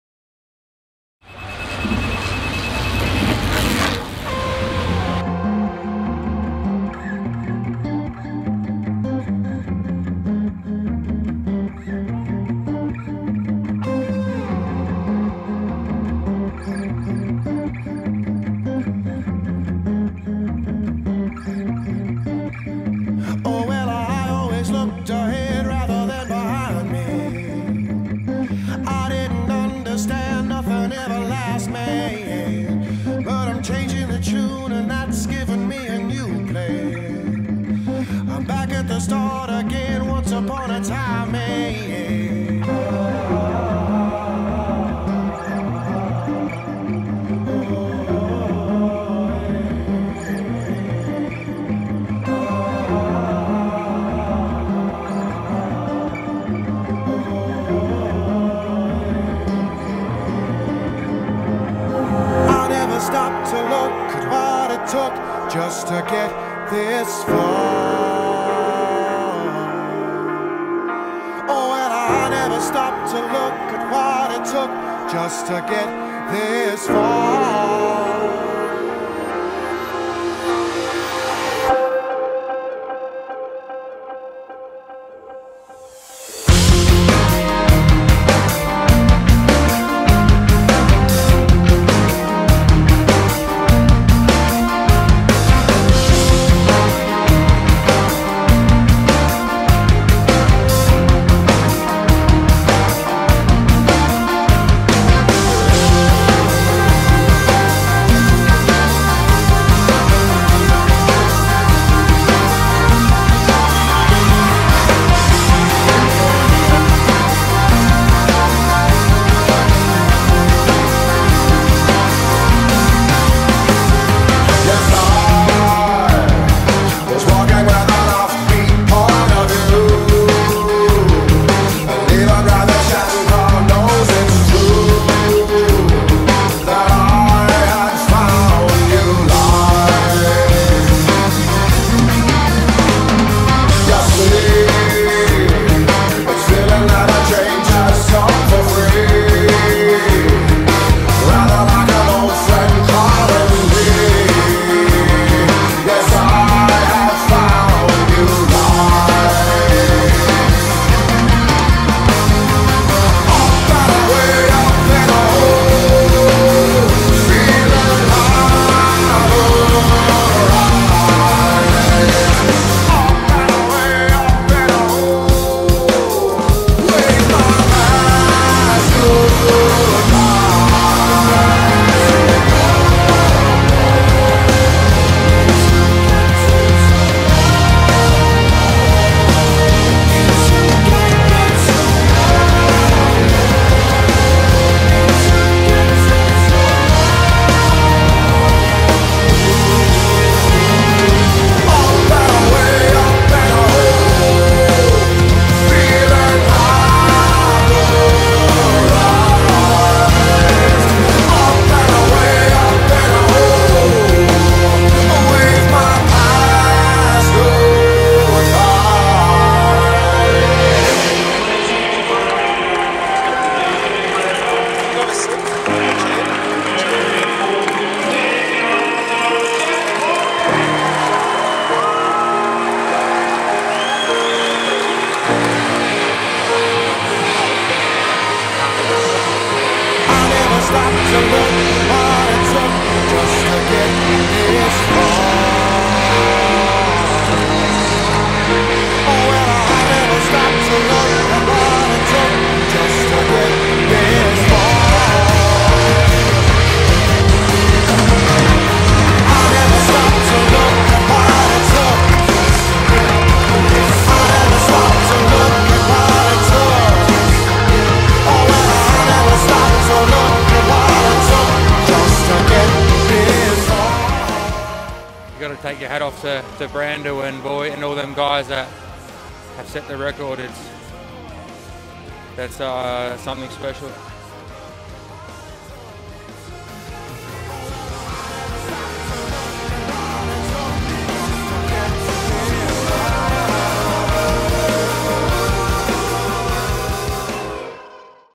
Jack Bobridge - Hour Record Team Budget Forklifts
Jack Bobridge of Team Budget Forklifts goes 51.3km to become the second fastest rider in history of the hour record. At the DISC velodrome in Melbourne on the 31st of January 2015. Video by Korupt Vision.